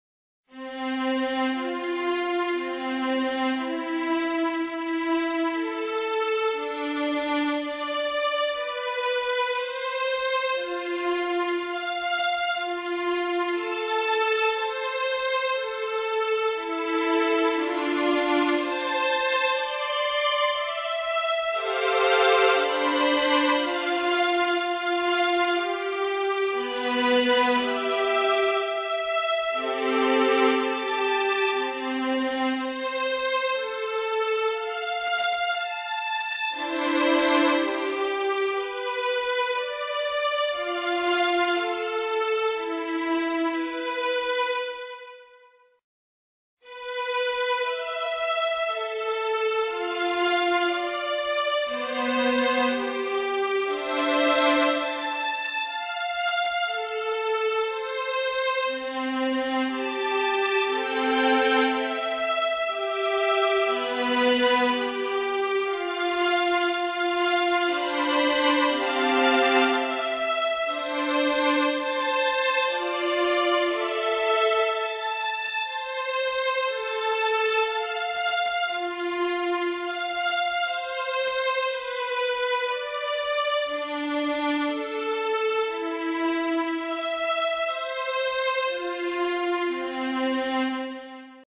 Música del manto de la Virgen de Guadalupe
Modelo matemático de Fernando Ojeda para dibujar las notas musicales de arriba hacia abajo